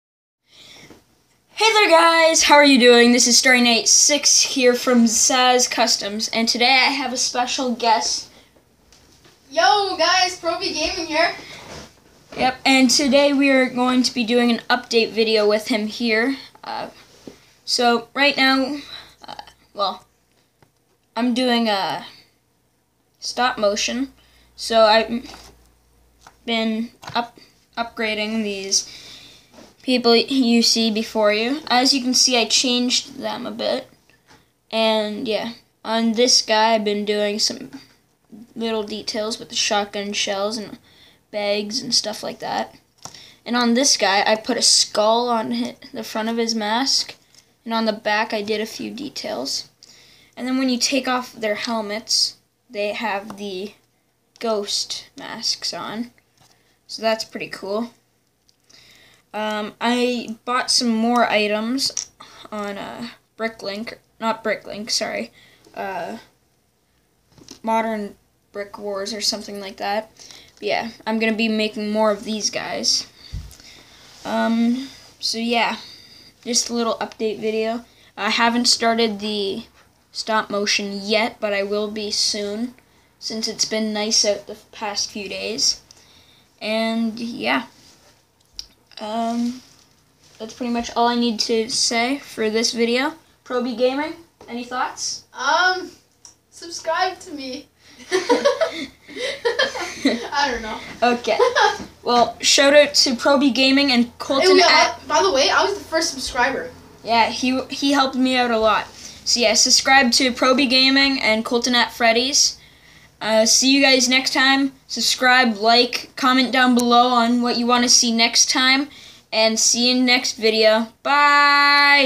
Pro B gaming is my friend?
Hey guys today I did a collab with pro B gaming so please show them the love you show me and sub to pro B gaming  and Colton at Freddys